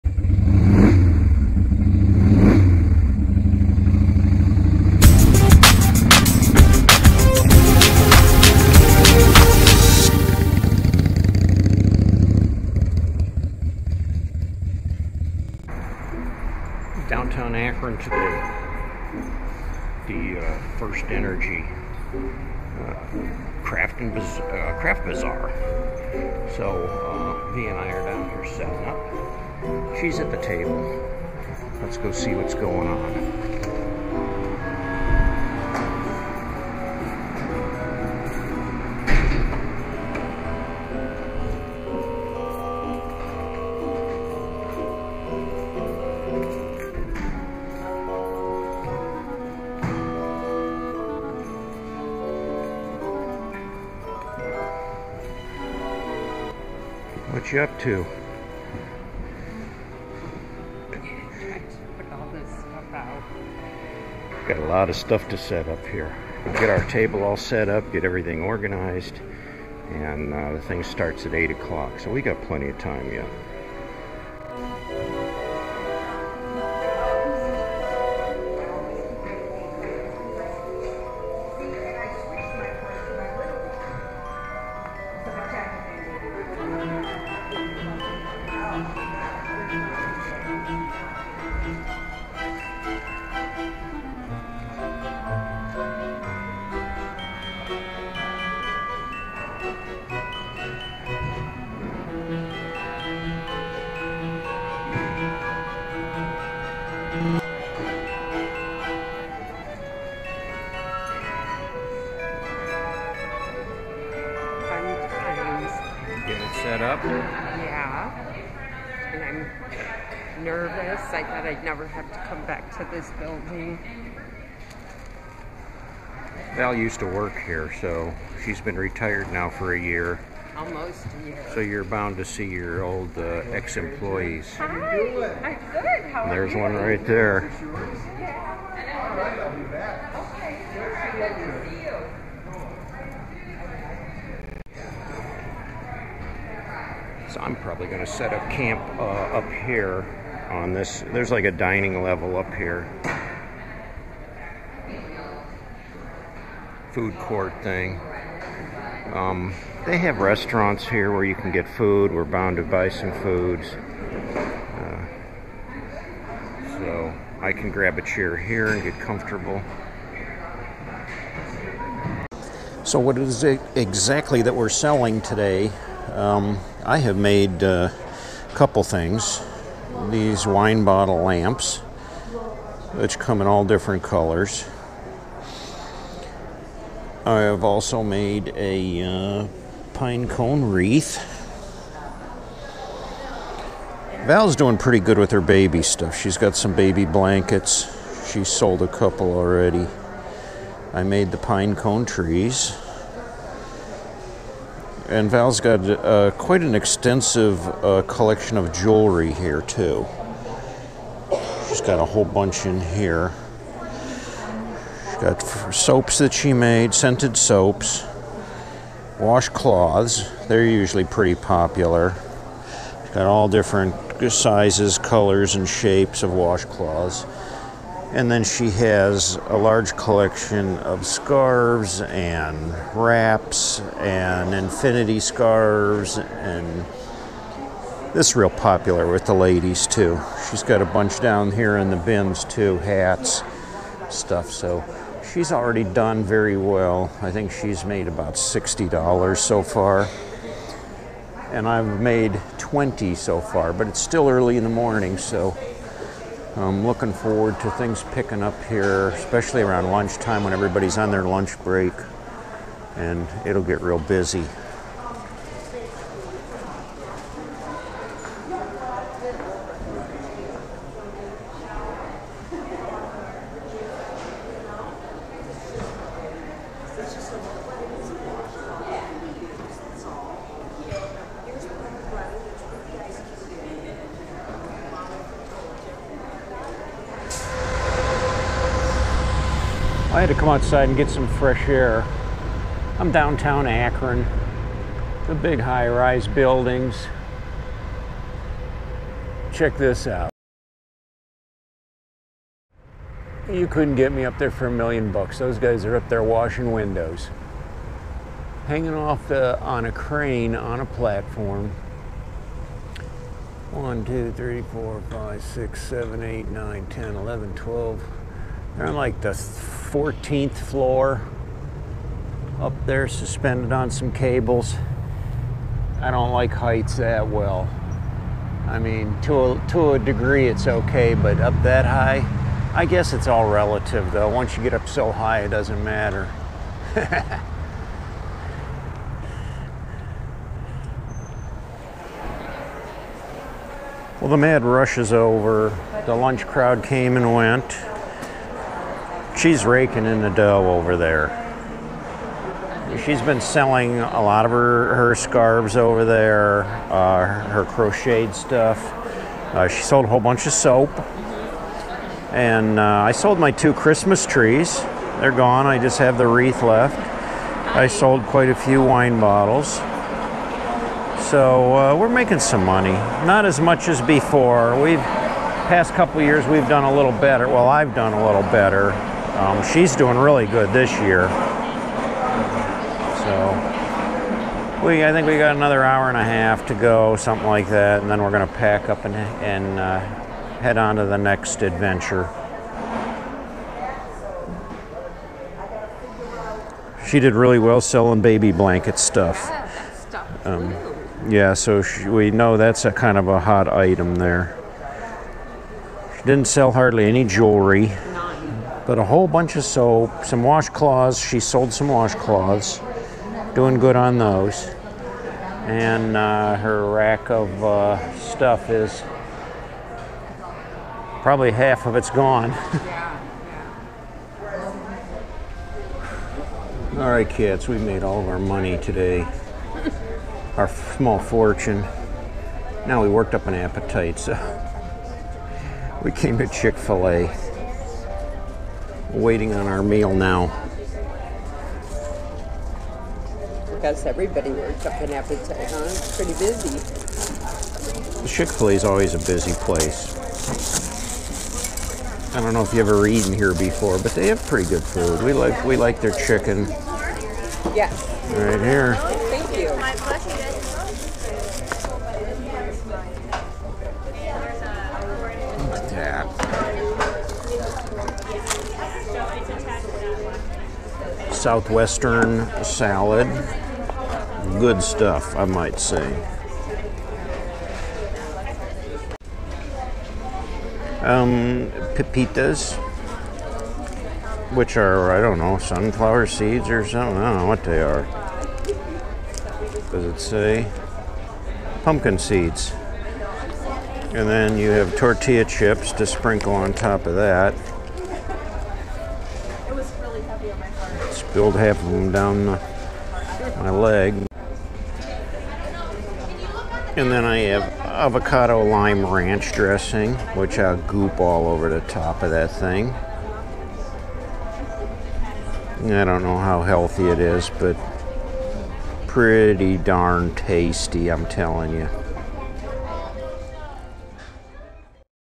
How to make easy money! - the Craft Bazaar
The annual craft bazaar at First Energy building's Orangery Mall in downtown Akron, November 2016. Itchy and Vee offer a few of their hand made goods for sale.